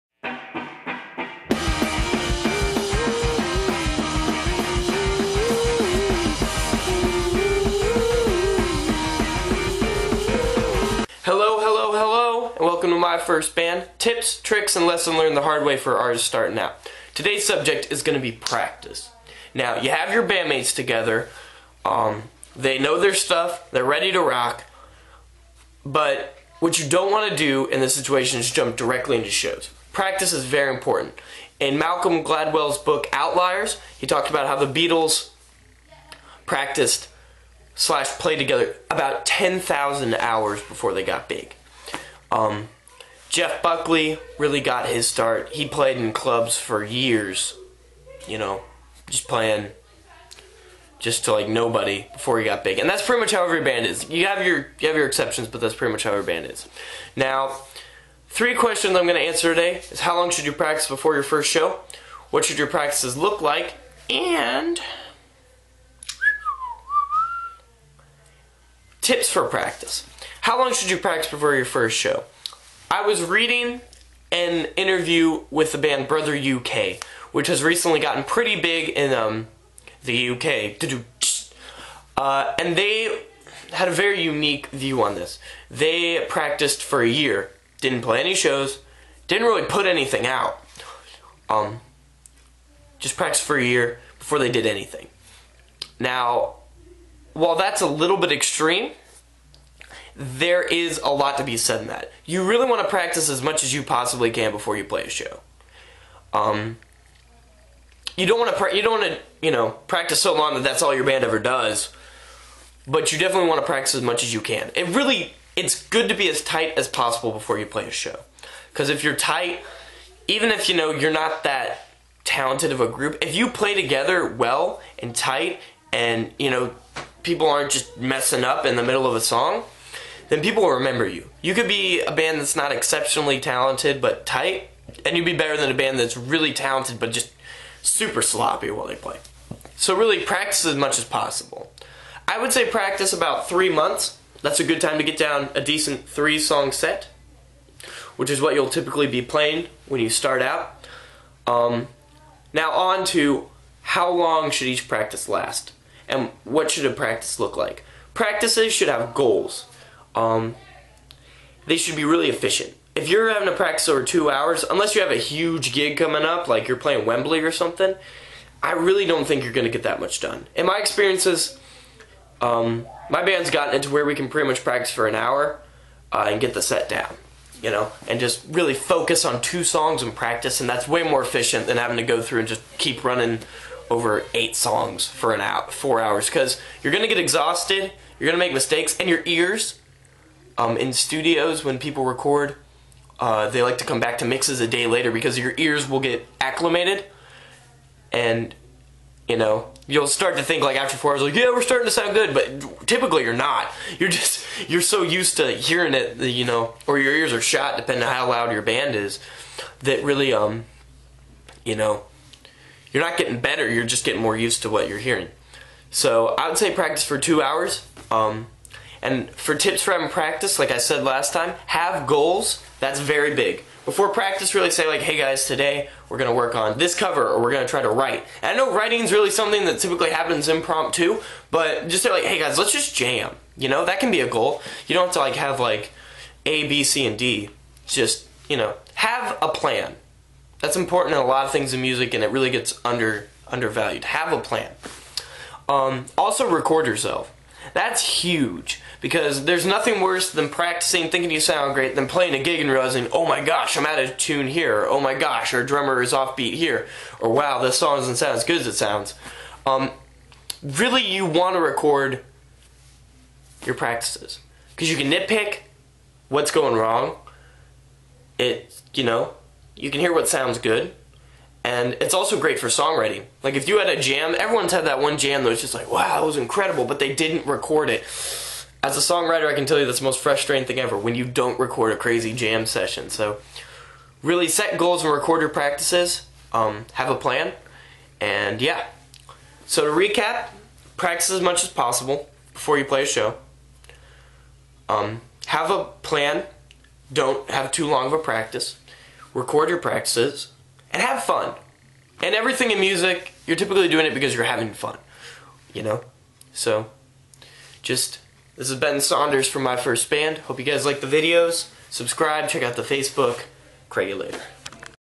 My First Band; Practice
Tips on Practice to help your band achieve their goals quicker and more efficiently.